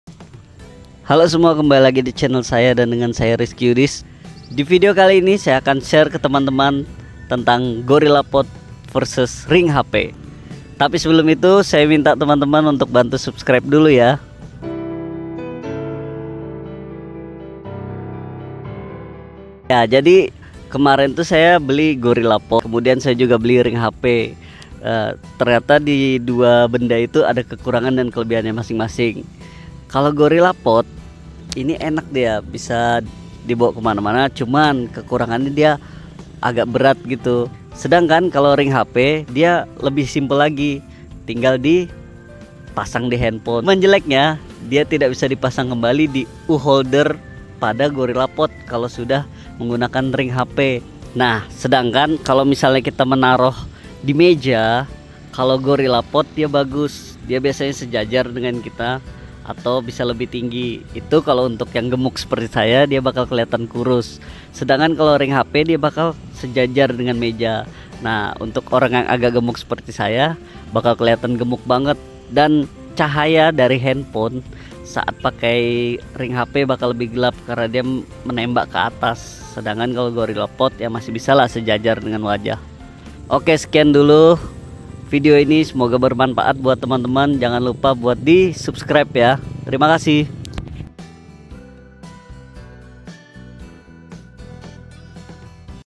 GORILLA POD VS RING HP : YOUTUBER PEMULA MODAL HP
Di video ini saya mereview gorilla pod vs ring light dalam hal kemudahan pemakaian dan cahaya juga angle.